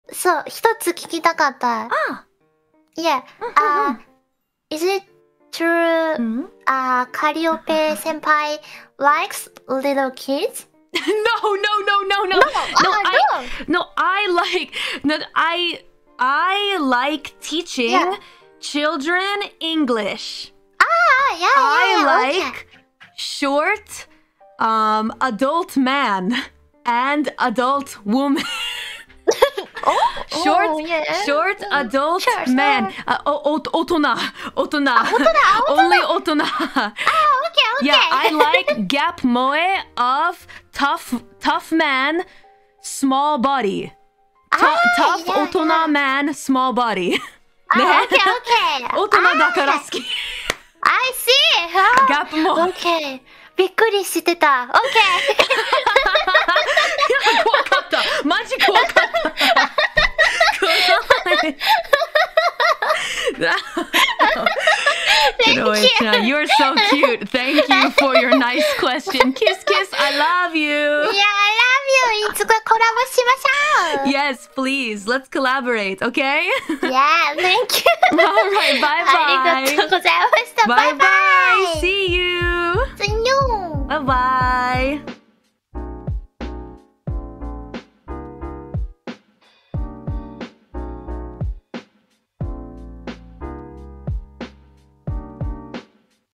Chloe asking Calli DANGEROUS QUESTION【HololiveEN clips】【Sakamata Chloe】【Mori Calliope】
Chloe asking Calli yabai question about little kids.